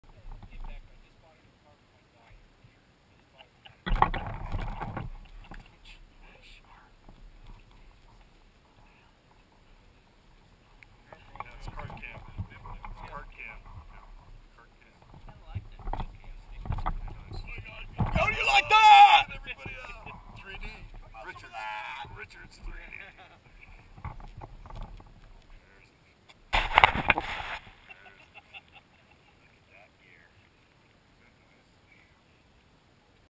DannyO'GetUp
Dan Richard O'Mara goes vintage on the first tee at the Swingin' Richards preseason event at Mick Riley Golf Course on April 20, 2011. He played his round with 1940's & 50's era clubs. Now that's what a Richard would call Snappy. After the round he said he shot the same score as he does with his modern day Hi-Tech Clubs. Kevin Richard Stitt gets in on the act.